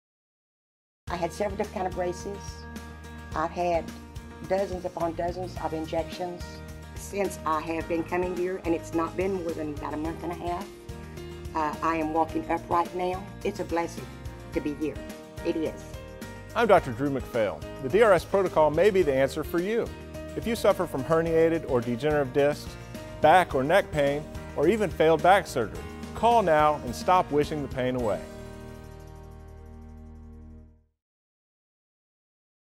MCPHAIL DRS
We're having phenomenal results with our DRS Protocol. See what our patients have to say. To see our full line of treatments go